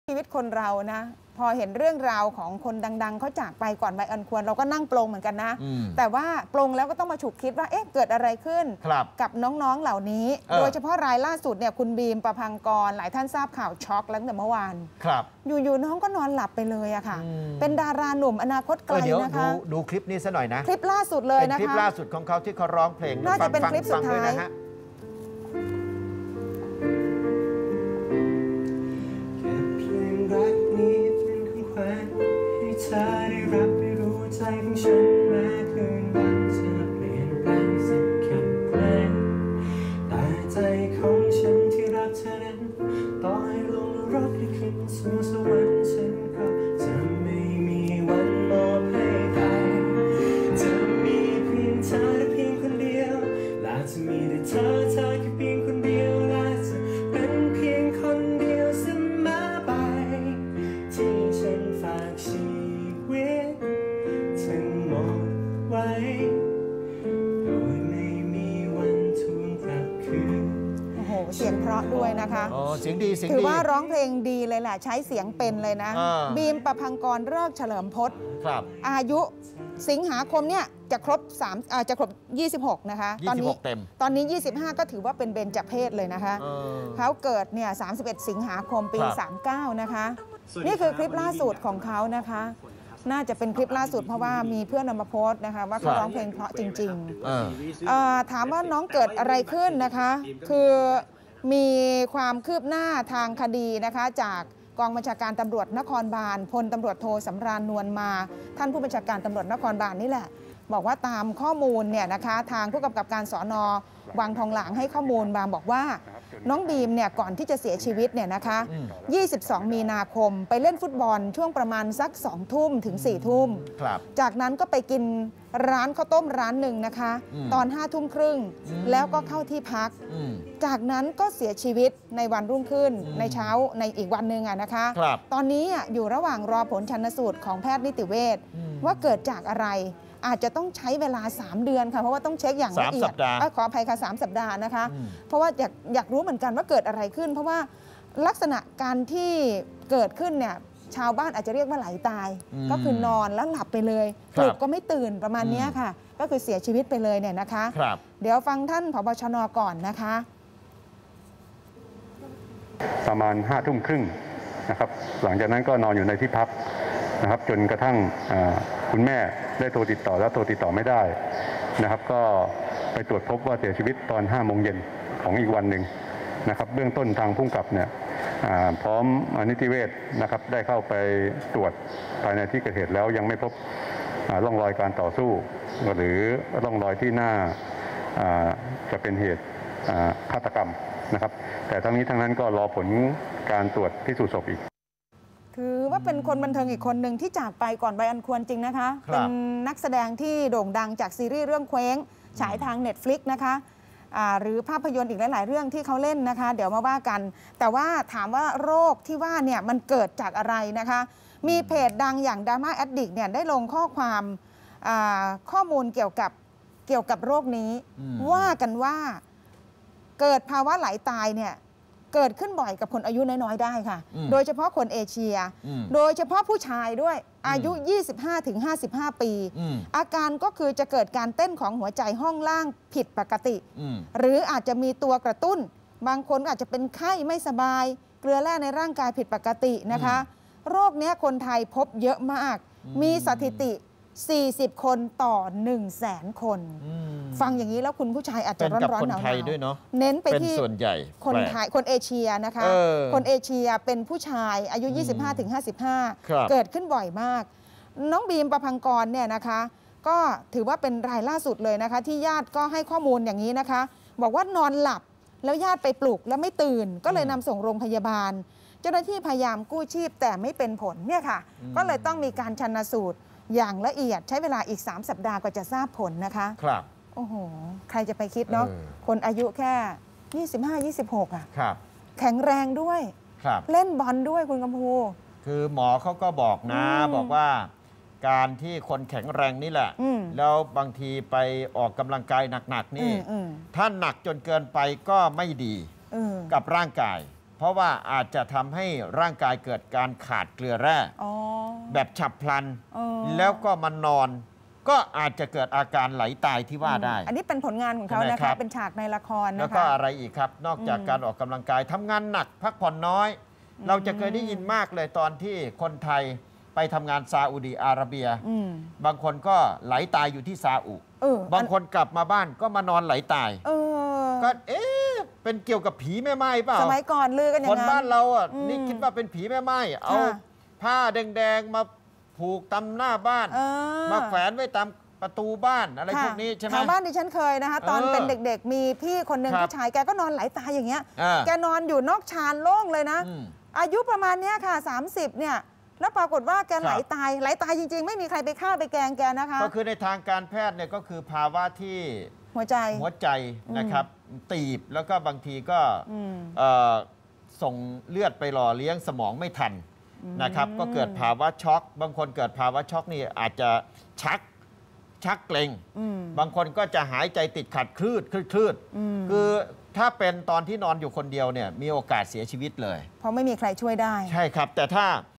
แพทย์ชี้ “บีม-ปภังกร” เกิดภาวะ ‘ใหลตาย’ พบบ่อยในคนเอเชีย
จากกรณีนักแสดงหนุ่ม “บีม ปภังกร” เสียชีวิตขณะนอนหลับ ซึ่งญาติเร่งนำส่งโรงพยาบาล และแพทย์พยายามกู้ชีพเเล้วแต่ไม่เป็นผล ทั้งนี้ รอผลชันสูตรโดยละเอียดจากแพทย์นิติเวชประมาณ 3 สัปดาห์ เบื้องต้นแพทย์คาดว่า บีม เกิดภาวะ ‘ใหลตาย’ ซึ่งพบบ่อยในคนเอเชีย โดยเฉพาะผู้ชายอายุน้อย